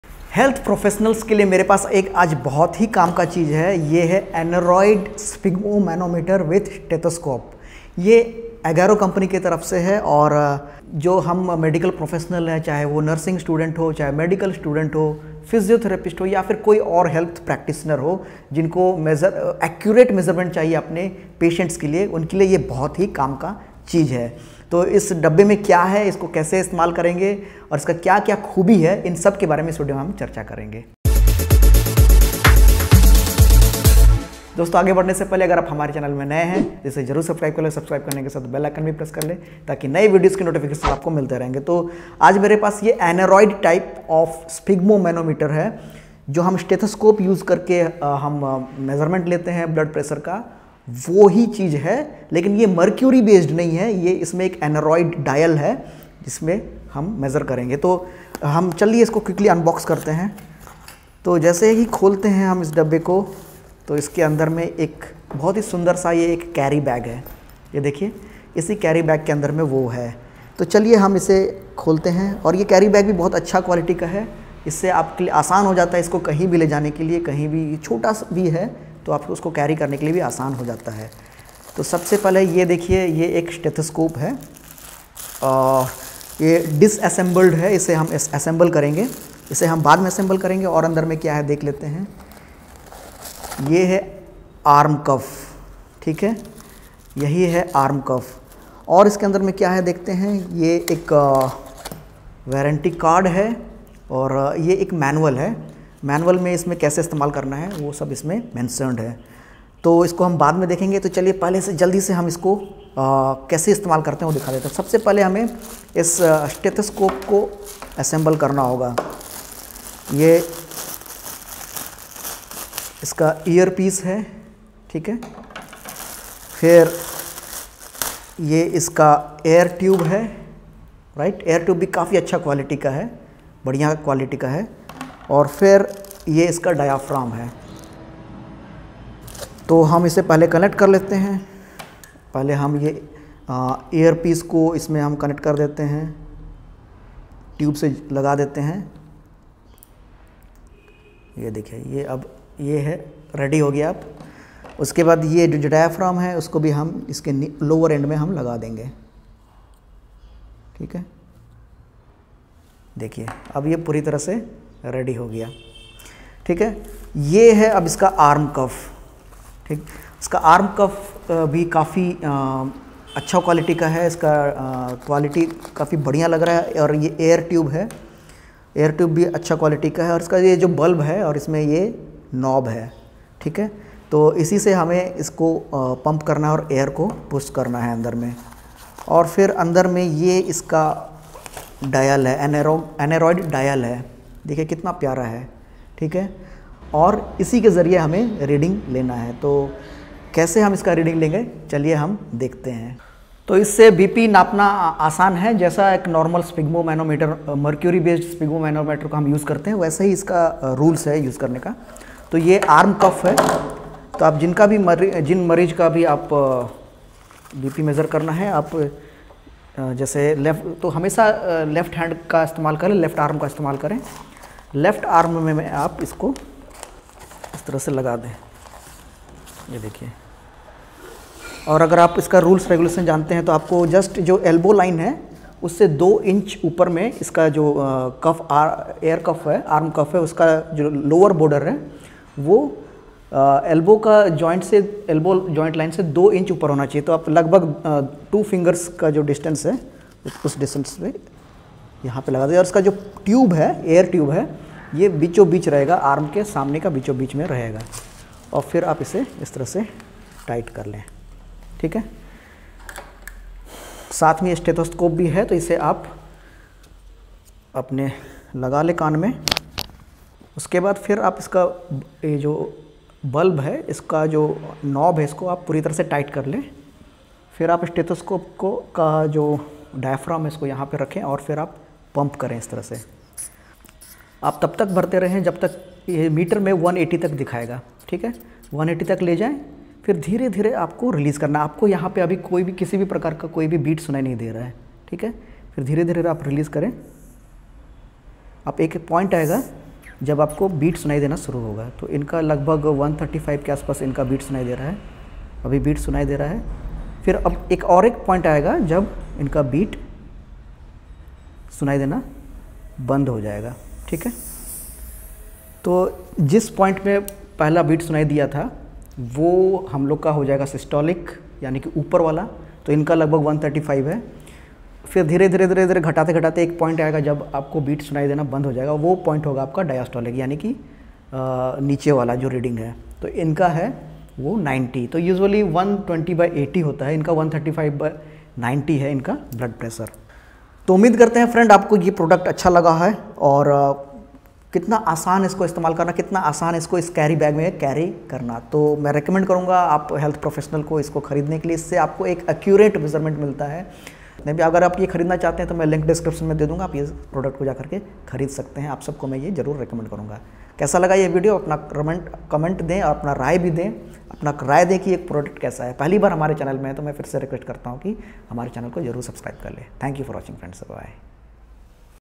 How to Use Aneroid Sphygmomanometer to Take Blood Pressure Accurately
Thank you Agaro for sponsoring this video, Buy Agaro Aneroid BP monitor today (Amazon)
In this video you'll know how to use Agaro aneroid type sphygmomanometer with stethoscope to take blood pressure accurately. Nowadays, high blood pressure is a common problem, so it's important to measure it correctly. There are some significant concerns about aneroid BP machine accuracy, and today you'll see how to check blood pressure using Agaro sphygmomanometer properly.